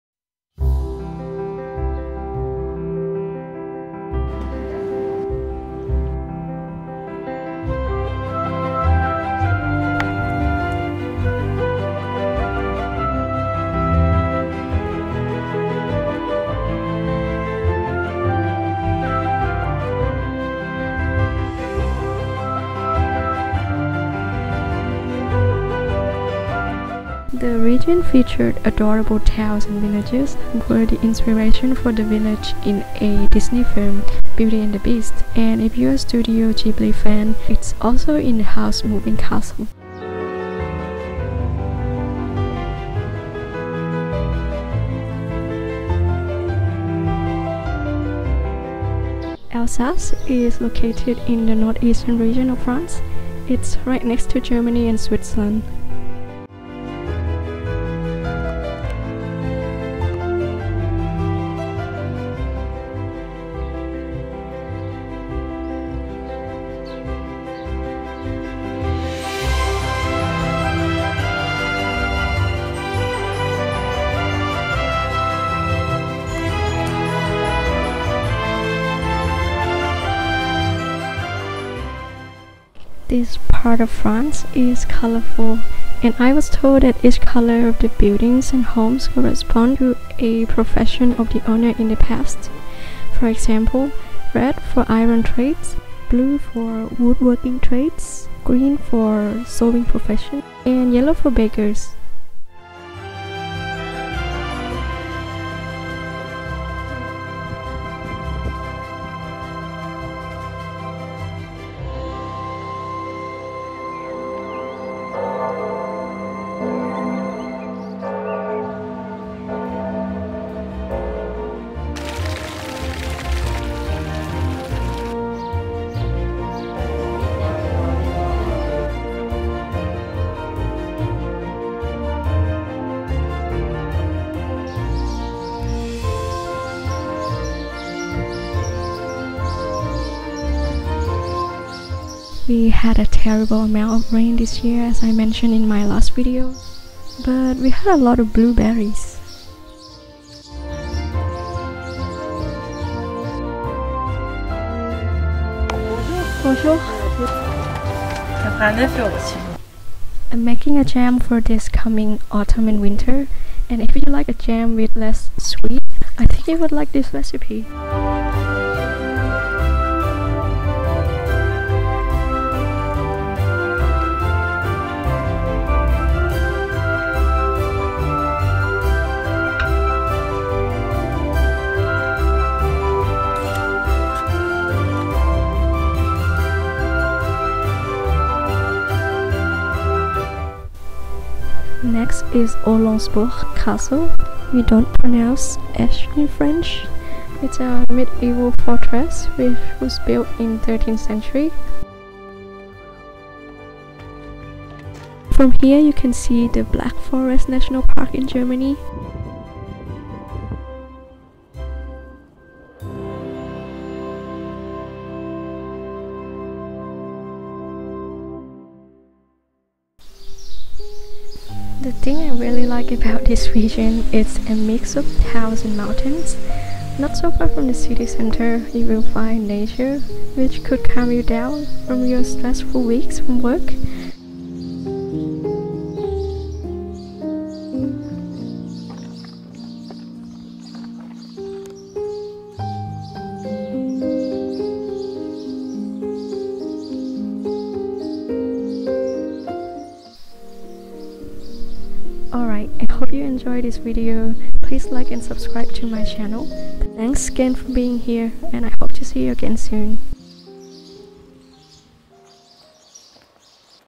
Simple Life in France's Fairytale Region | You won't believe this place exists | Small Town Vlog
In this video, I'm sharing a glimpse of my adorable region Alsace, France.

0:00 Intro
0:16 Colmar, France
1:11 Turckheim, France
1:54 Kaysersberg, France
2:16 Blueberry picking
2:39 Blueberry Jam Recipe
3:12 Castle of Hohlandsbourg
3:40 Hiking nearby

Ps. I apologise for the quality of the voice over that you might notice. I'm still learning to get better at voice recording. It will improve in the next videos.

References :
Colmar

Equipment:
Camera: Fujifilm X-T30
Editing suite: Davinci Resolve 17